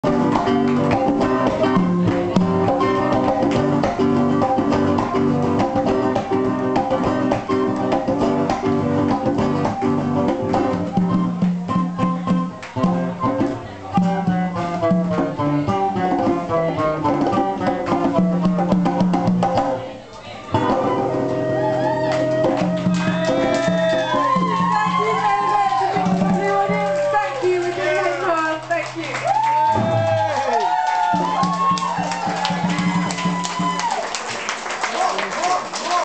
The Hidden Half @ Mr Elephants Live & Free 3 @ The Adam & Eve - Sat 20th June 2009
The Hidden Half performing at Mr Elephants Live & Free 3 @ The Adam & Eve - Sat 20th June 2009